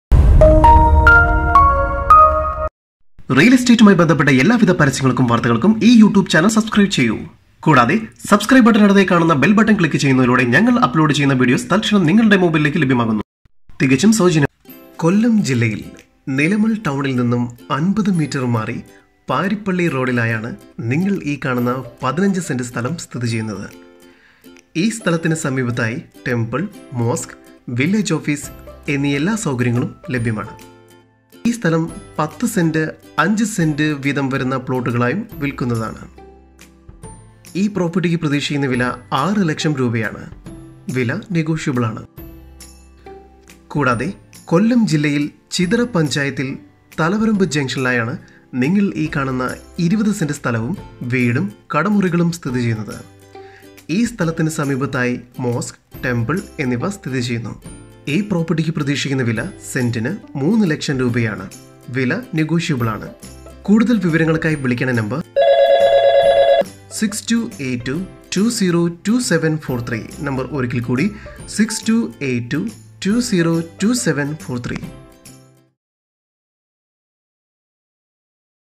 Plot For Sale In Kollam | Nilamel | Kollam Properties | Owner +91 6282202743
PlotForSaleInKollam,

Plot For Sale In Kollam  | Nilammel | Kollam Properties |
15 Cent Plot For Sale
Price On Property 6 lakhs Per cent
another Plot
20 Cent With house ,shutter rooms
Price 3 lakhs Per cent
For More Details
owner name : Mr JAWADKHAN

Published by

JUS MEDIAS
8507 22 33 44